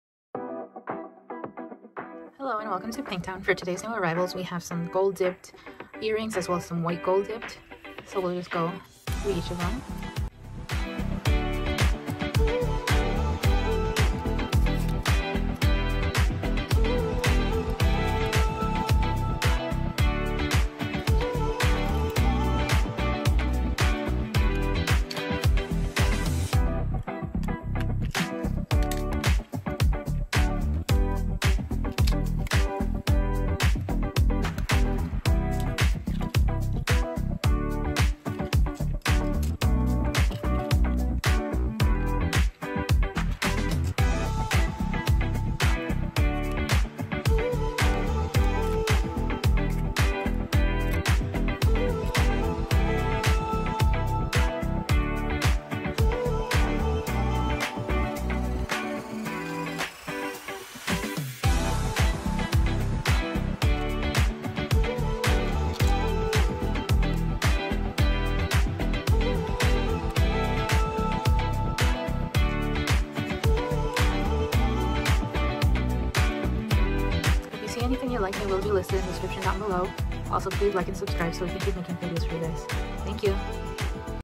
Unveiling Stunning 14K White Gold & Gold Dipped Earrings
Don't miss out on the chance to elevate your jewelry offerings or enhance your personal style with these stunning earrings.
Discover the allure of 14K White Gold and the charm of Gold Dipped Earrings in this comprehensive review.